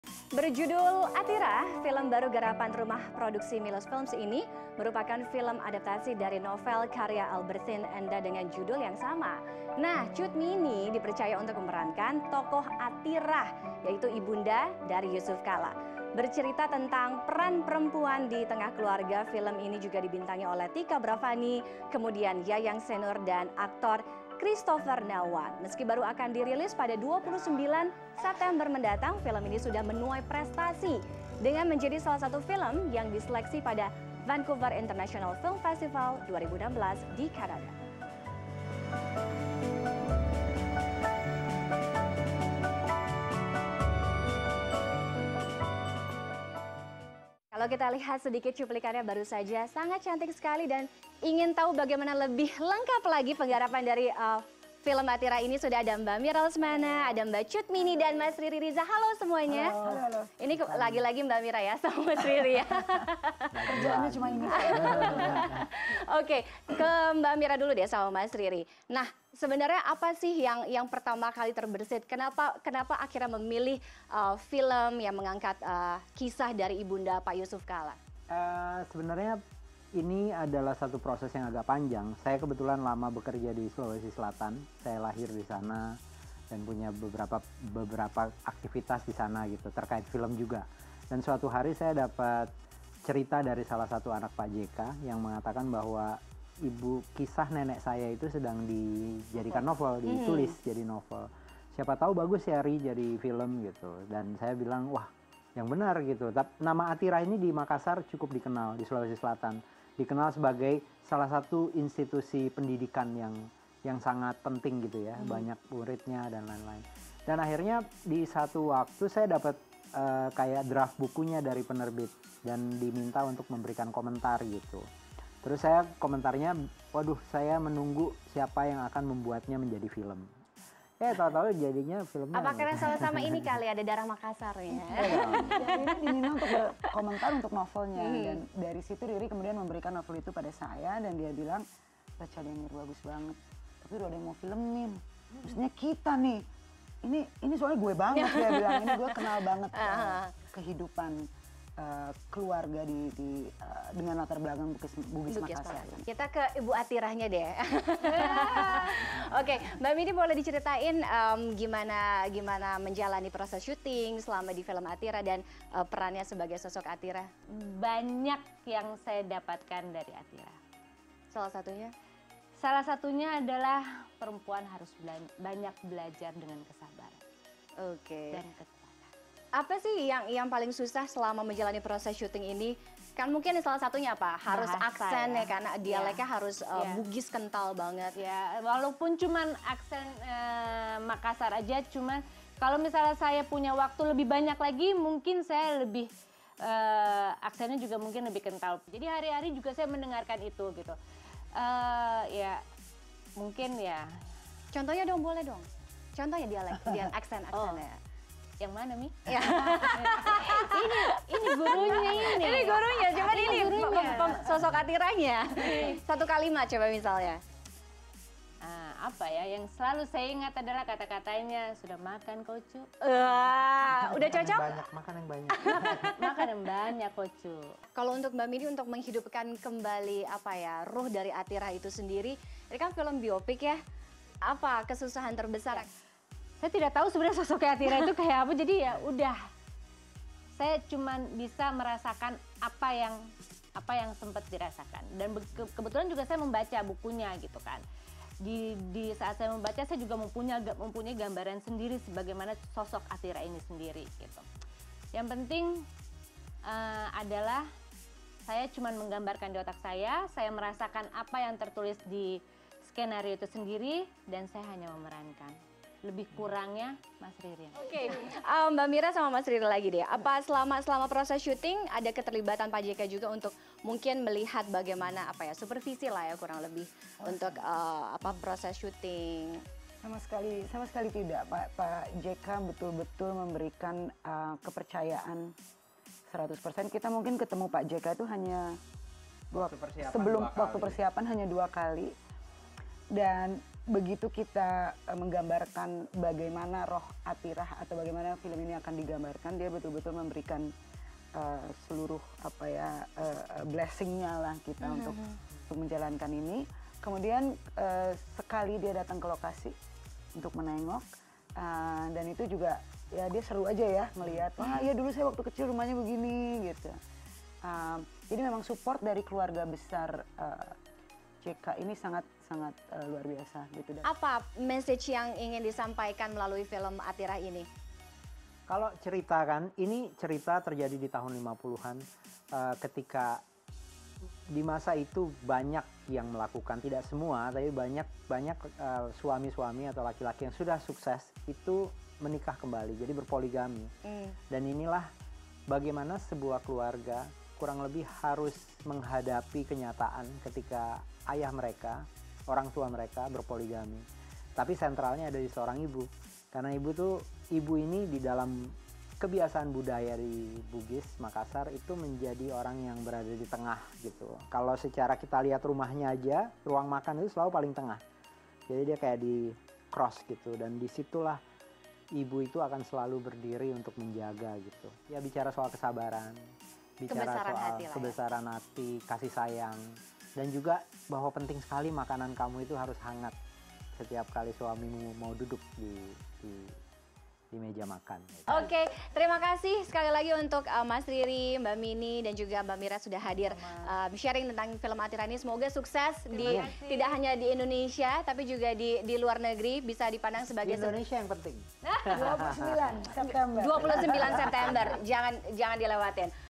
Showbiz News: Wawancara bersama Sutradara, Produser & Pemain Film "Athirah"
Simak wawancara Showbiz News bersama Mira Lesmana, Riri Riza dan Cut Mini yang terlibat dalam pembuatan film baru yang menceritakan sosok ibunda Jusuf Kalla, Athirah.